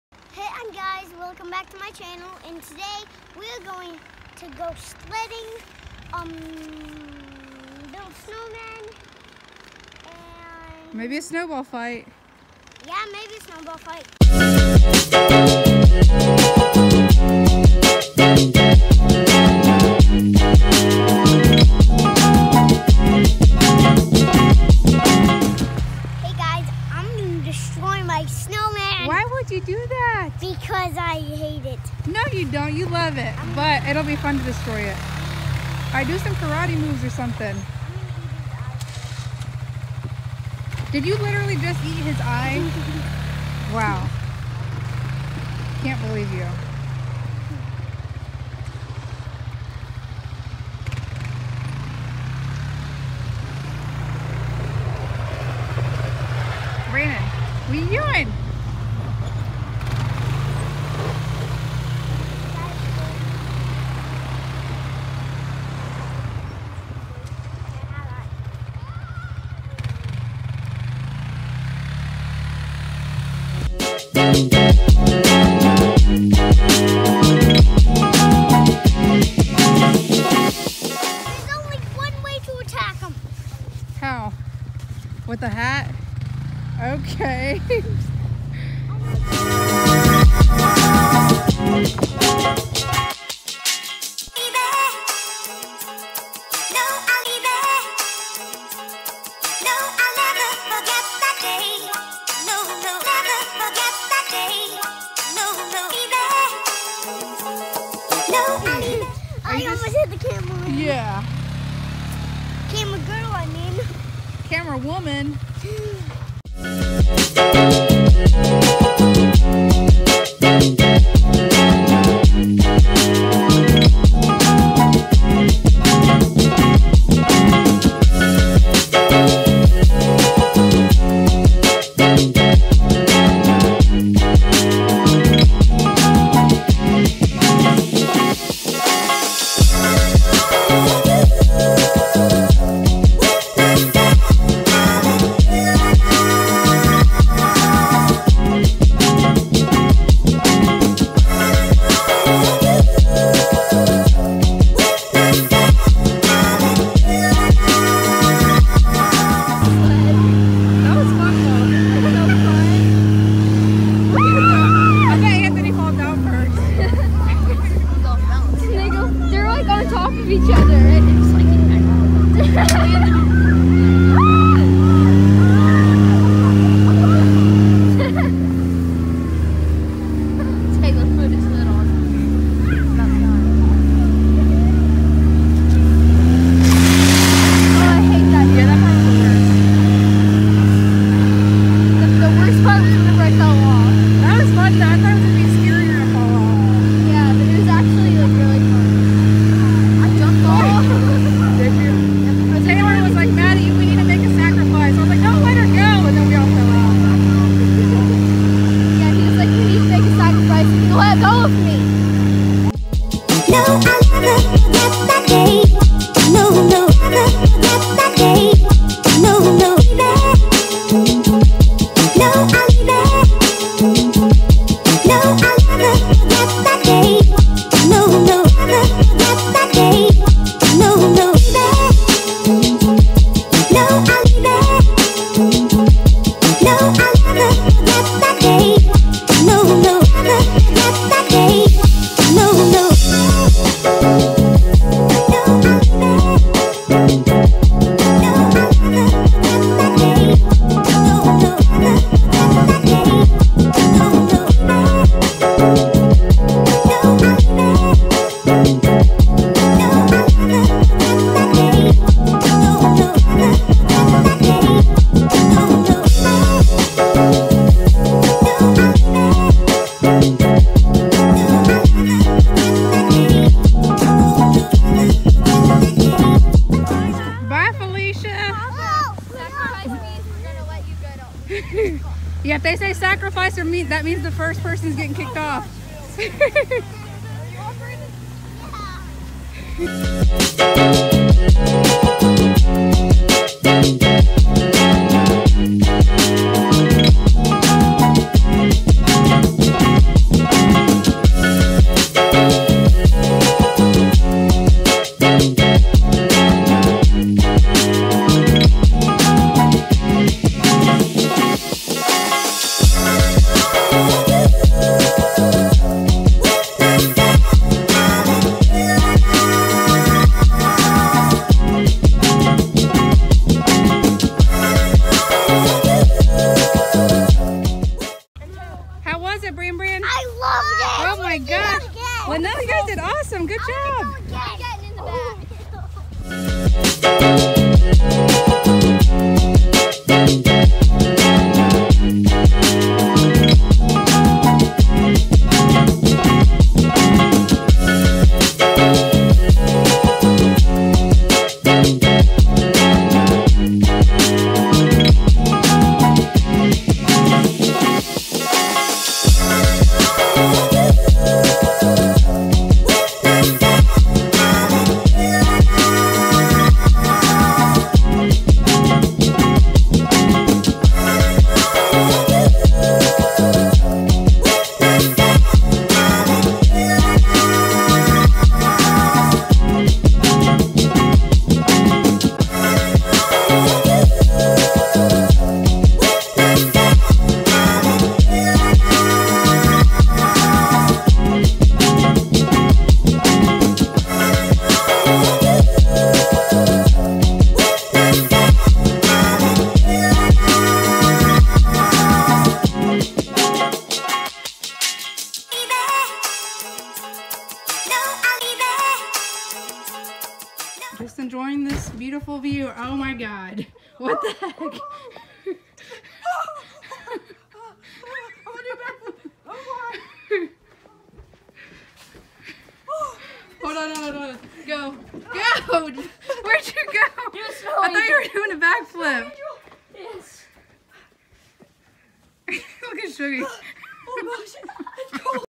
Perfect snow day!!
Video of our perfect snow day Jan. 2019!

Musician: Jef